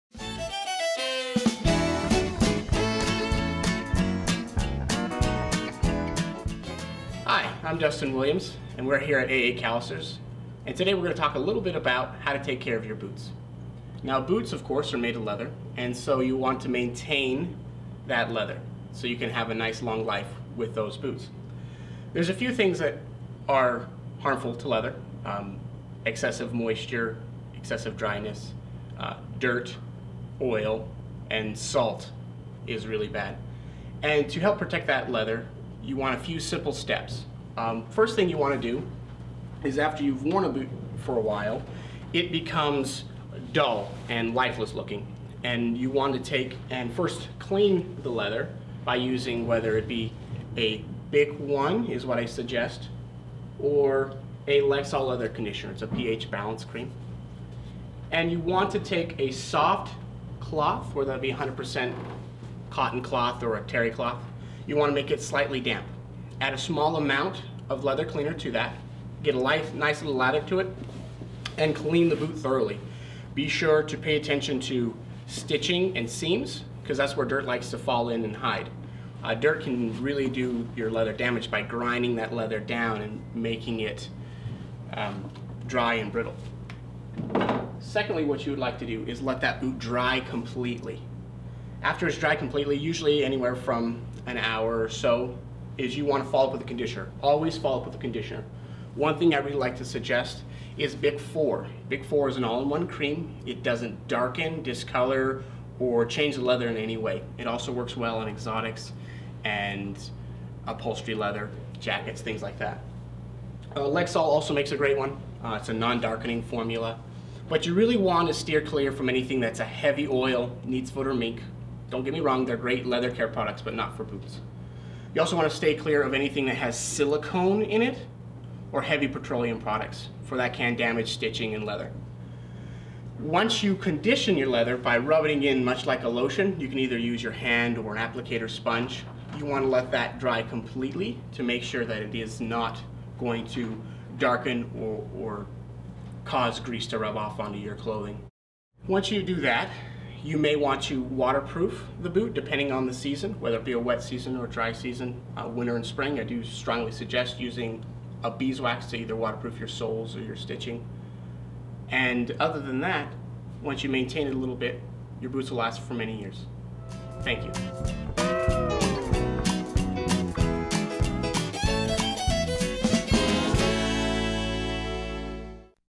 General Cowboy Boot Care
What to do and what not to do when caring for your cowboy boots.